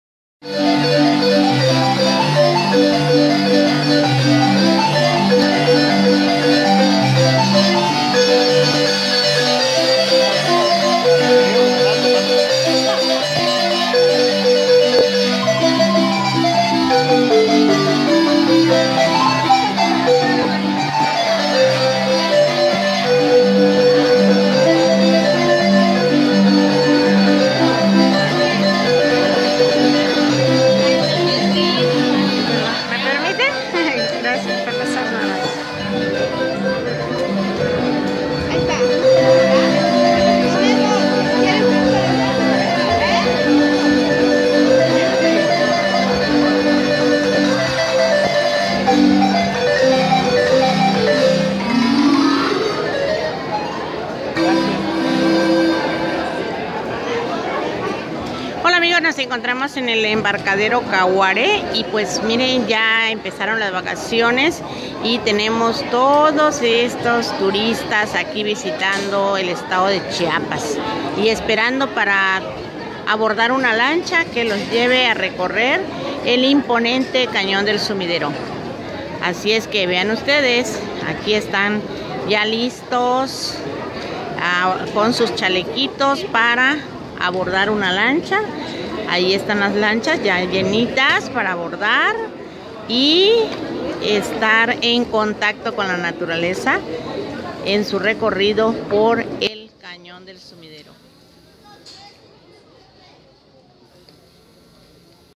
CAÑON DEL SUMIDERO
Embarcadero cahuare donde abordas una lancha y recorres el Rio Grijalva para admirar el imponente Cañon del Sumidero.